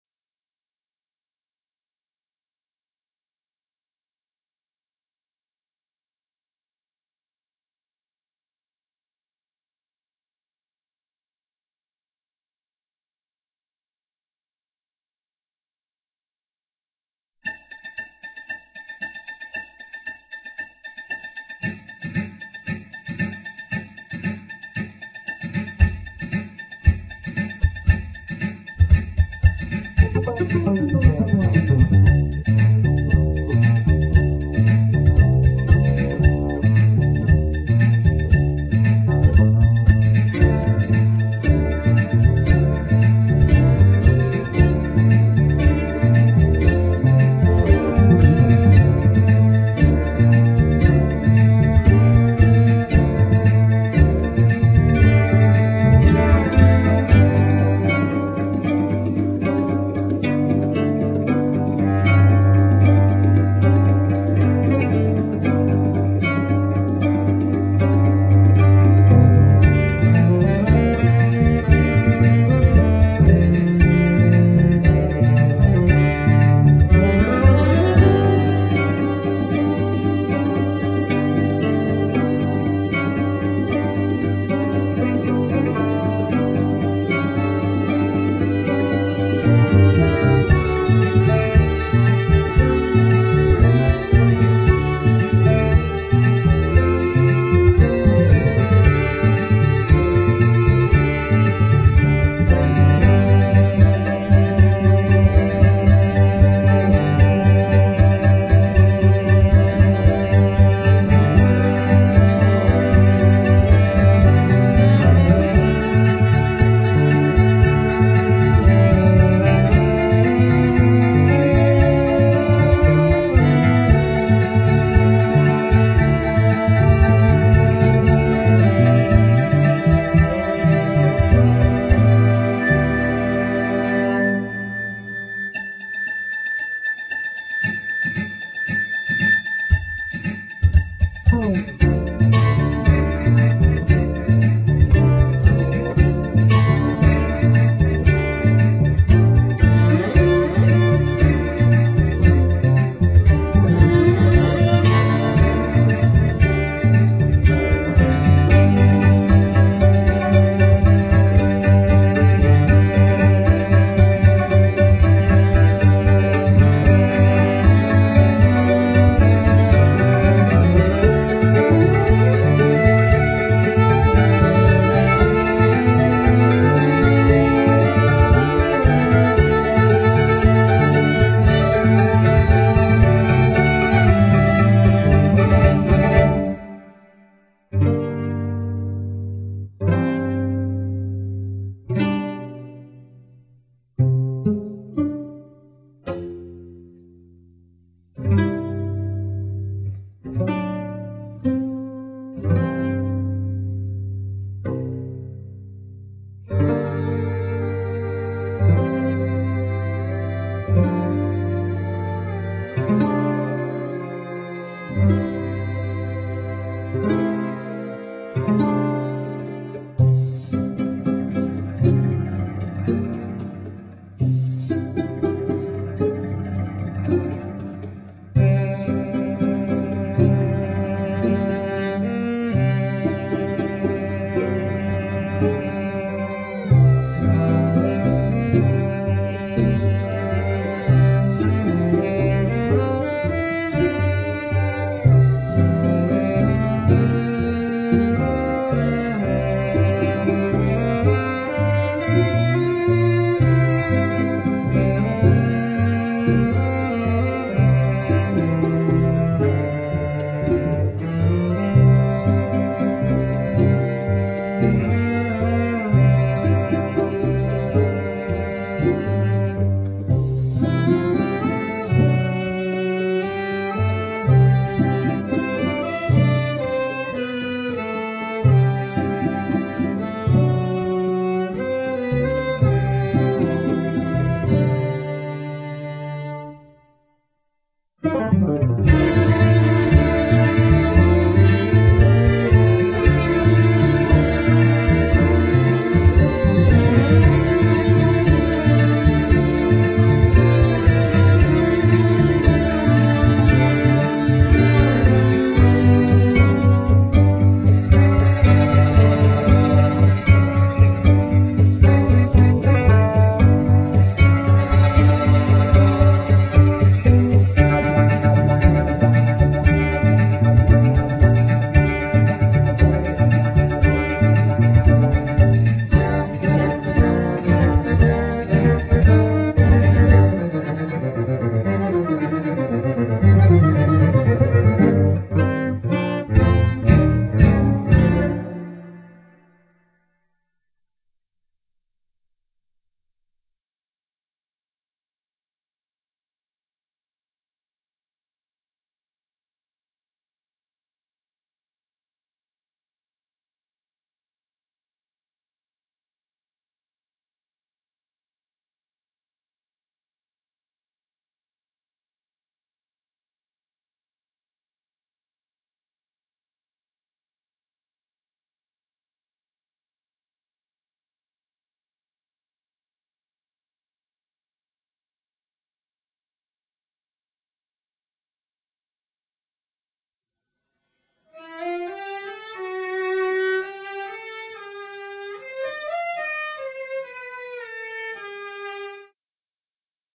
Amazing Video of a Song Composed Entirely of 37 Cello Parts
This is a pretty amazing video of Ethan Winer playing 37 separate cello parts to create one song. He even plays the percussion parts on his cello. It was recorded on 23 tracks using 37 plug-in effects. He spent hundreds of hours on this project so its worth a listen.